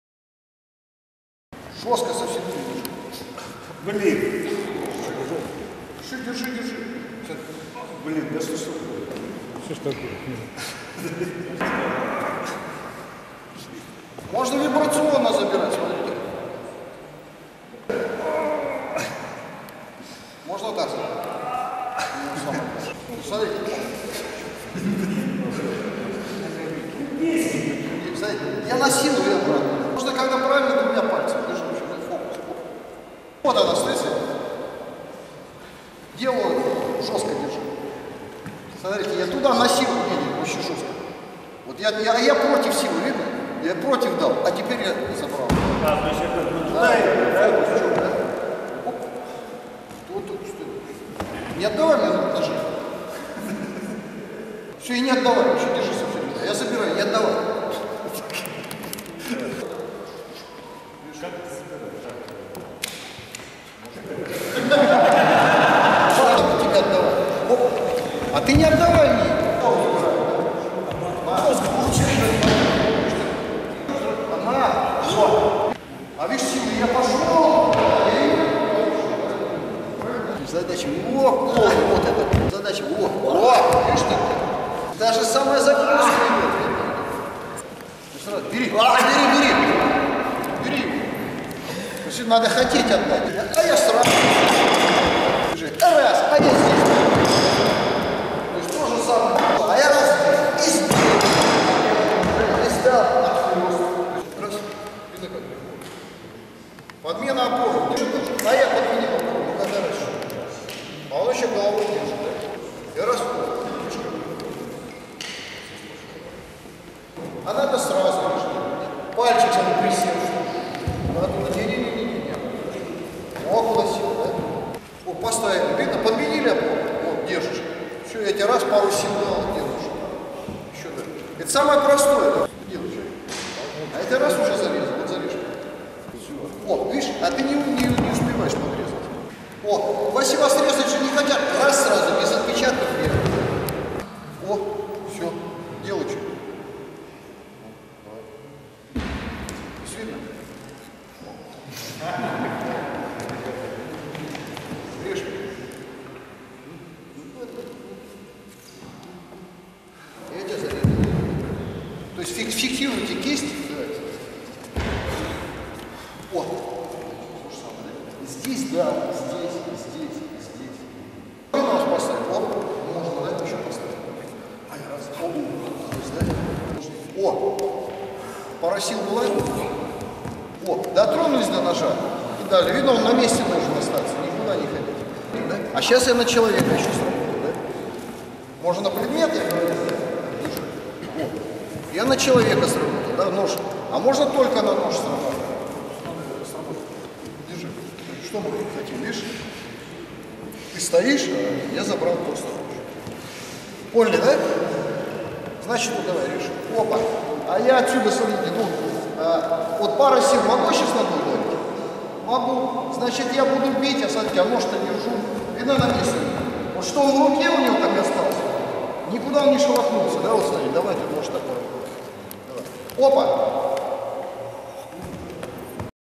Соловьев ОТОБРАТЬ и ВЫБИТЬ НОЖ СТАТИКА ДИНАМИКА ТЕХНИКА ОБУЧЕНИЕ ПИТЕР СЕМИНАР
Семинар Соловьева А . А . в Санкт - Петербурге по русскому стилю - системе русского боевого искусства . Отбор и выбивание ножа . Техника , статика , динамика , обучение